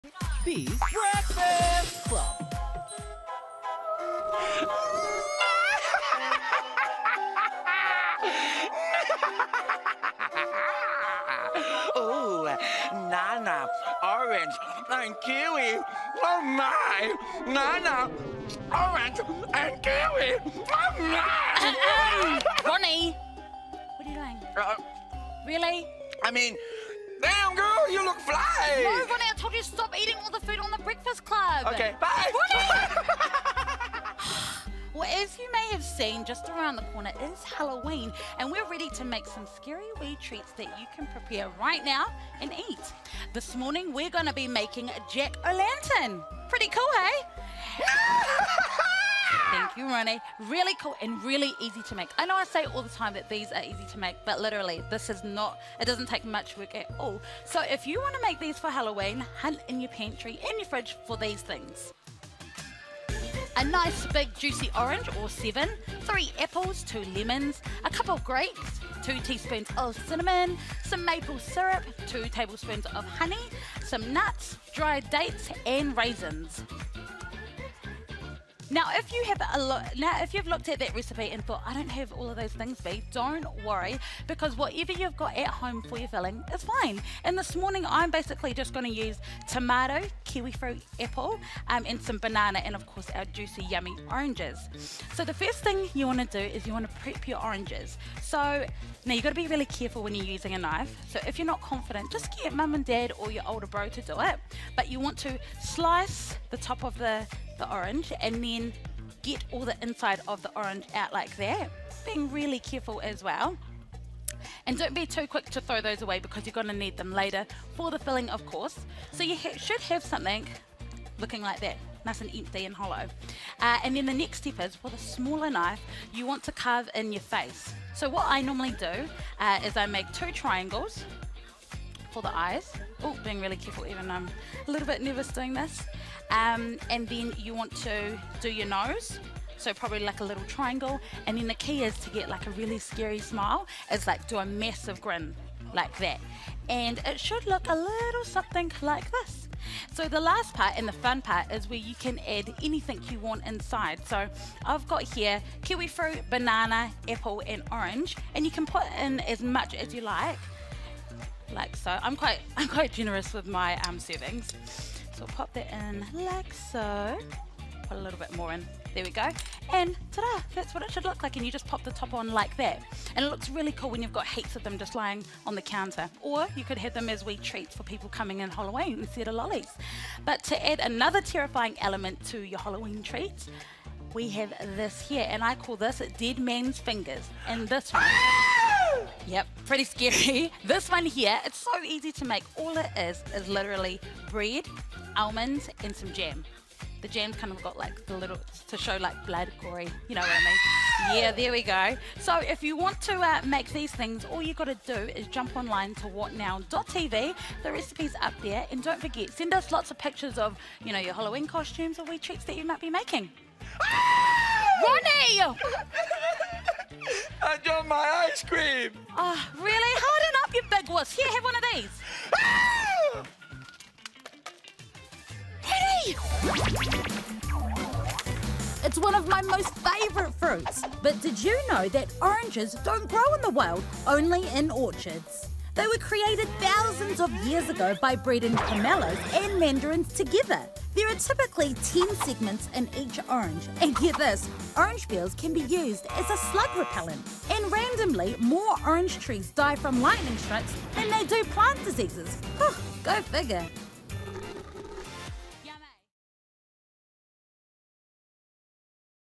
Orange Jack-o-Lanterns | Bee's Breakfast Club
Bianca shows us some spooky snacks!